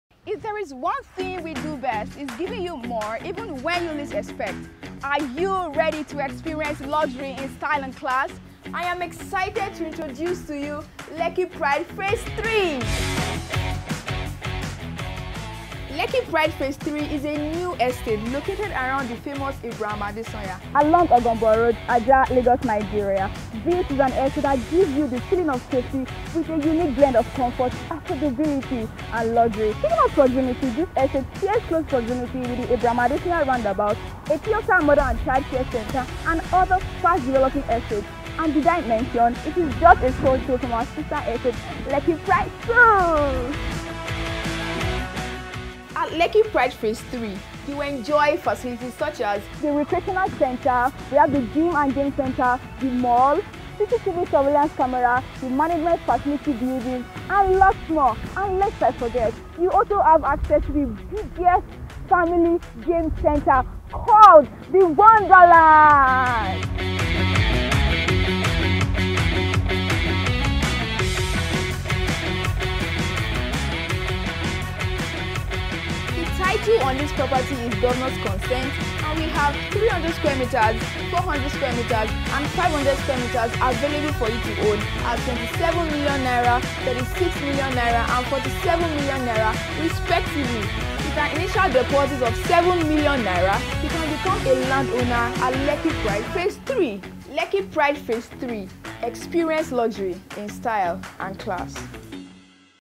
LEKKI PRIDE 3
Lekki Pride Phase III will offer exciting projects tailored to meet your highest standards, with excellently designed luxury homes, that bring comfort, luxury, and appreciation over time.

 It is an estate that gives you the feeling of safety with a unique blend of comfort, affordability, and luxury.

Experience luxury in style and class in an admirable estate designed just for you.  Lekki Pride III offers a blend of luxury and a serene ambiance. It is a unique and exquisite estate strategically built to solve the housing needs of Nigerians while meeting world-class standards.

Lekki Pride III shares proximity with:

Lekki Pride II
Dream City
Eti-Osa Maternal and Child Centre
Ajiwe Police Station
Conoil Filling Station
Agofure Motors
God is Good Motor Park
Abraham Adesanya Roundabout and many more notable landmarks.

Estate Features:

FACILITIES

Facility management Building
24-hours Power Supply
Drainage System
Central Water Supply System.
Electric Fencing
Gym and Game Centre
Security Gatehouse
Management Office Building
Cinema House
Recreational Centre
Solar Powered Streetlights
CCTV Surveillance Cameras
Pharmaceutical Store
Estate Gym
Solar Street light
Smart Home Features
Drainage system
Excellent drainage & accessible road
All room en-suite and BQ
Stainless steel staircase and Balconies
Spacious Parking Space

to book an inspection.